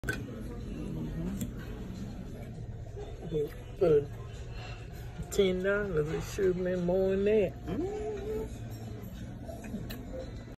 WOULD YOU EAT THIS?
WOULD YOU EAT THIS? BBQ STICKY RIBS AT RUBY TUESDAY’S